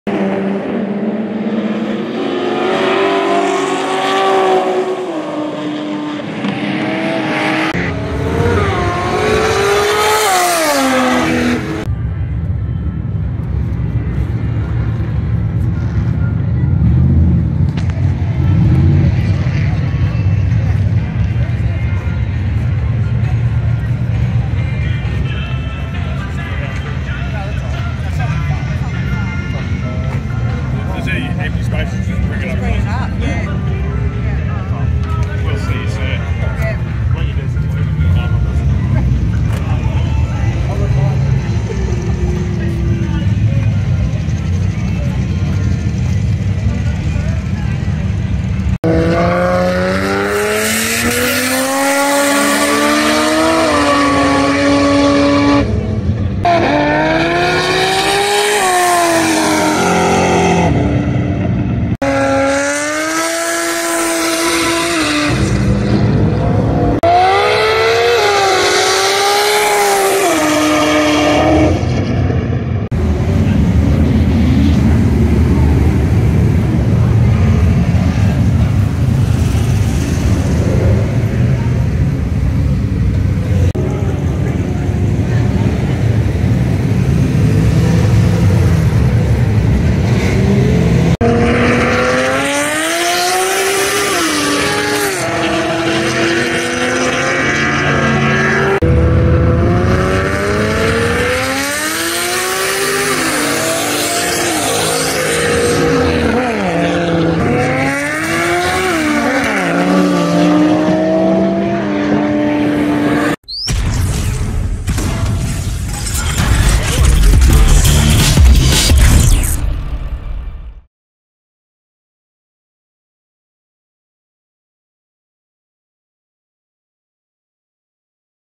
1200 HP TURBO BARRA POWERED BEDFORD VAN POWERCRUISE
1200 HP TURBO BARRA POWERED BEDFORD VAN POWERCRUISE QUEENSLAND RACEWAY 2022